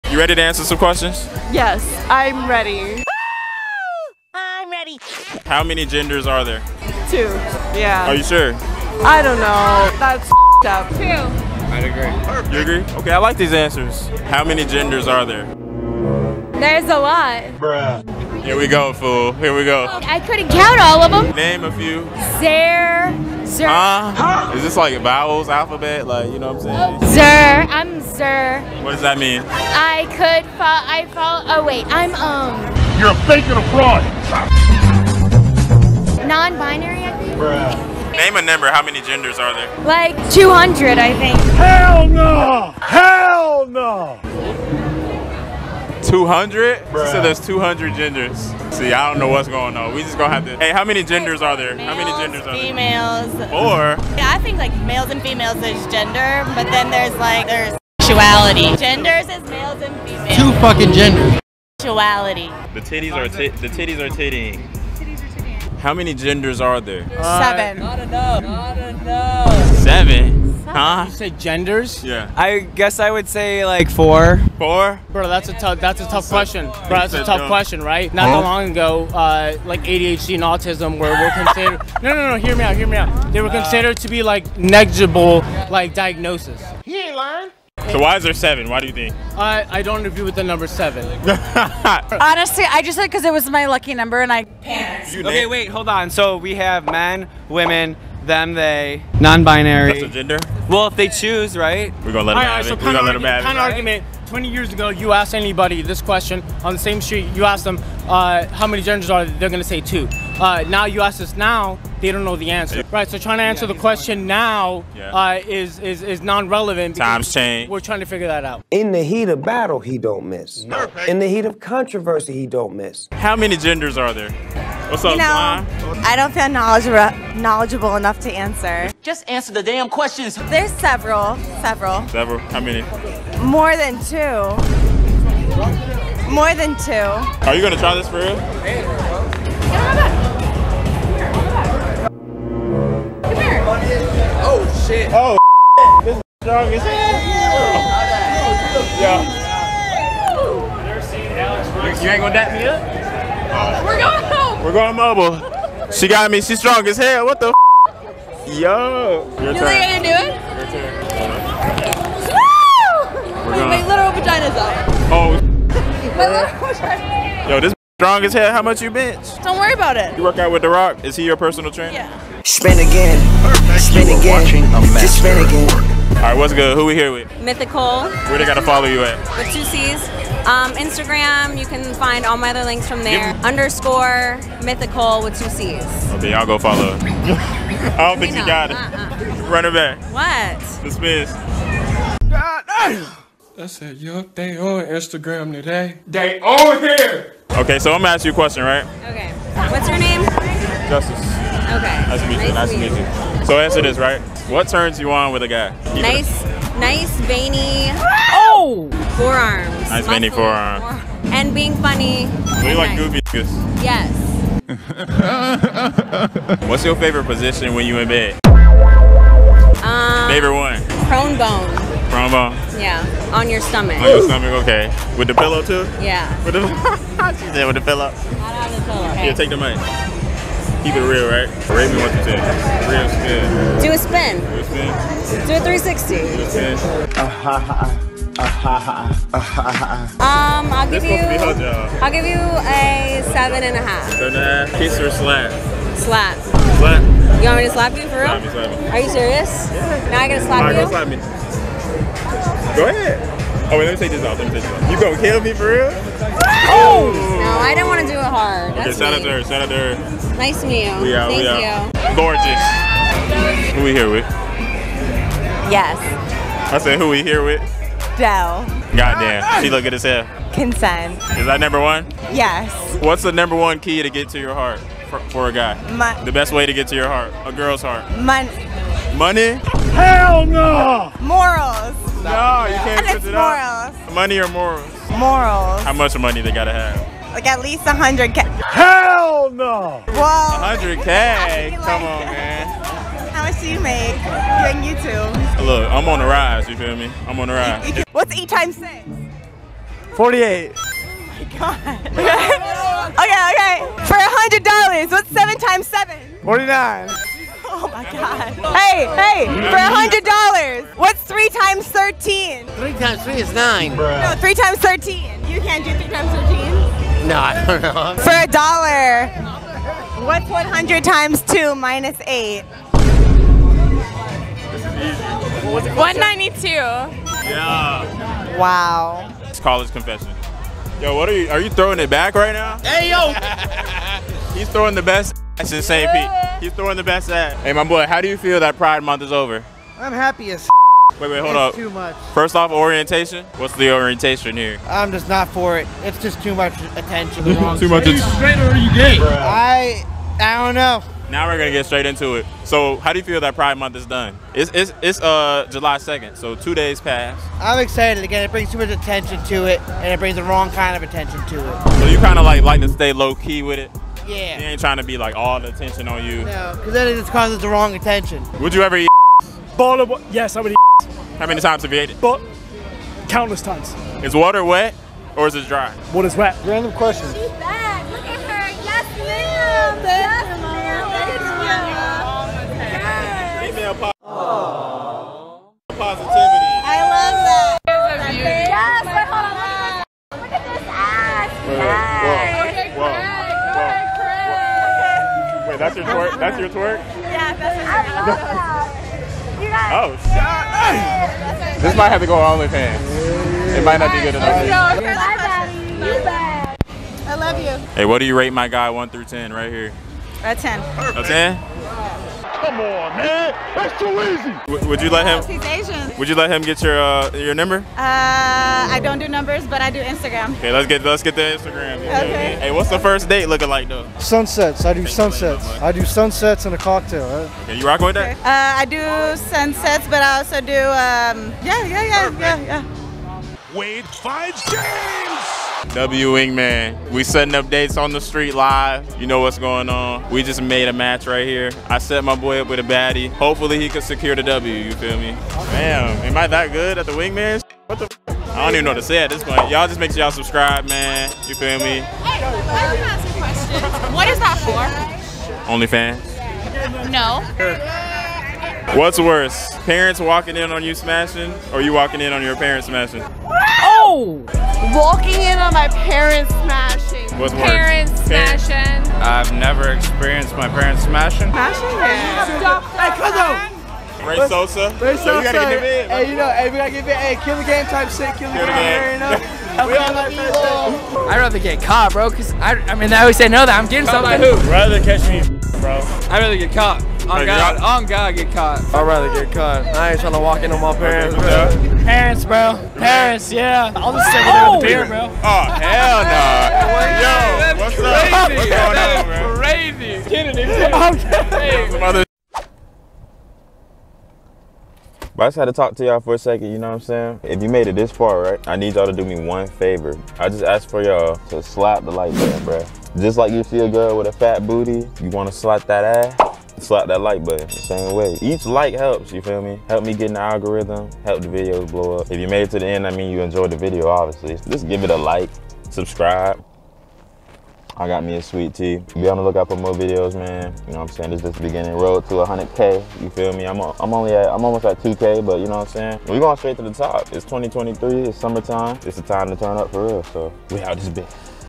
Asking people how many genders there are! (Public Interview)
Went back to downtown St Pete to hear peoples thoughts on how many genders there really is! St Pete pride parade just passed so it’s fitting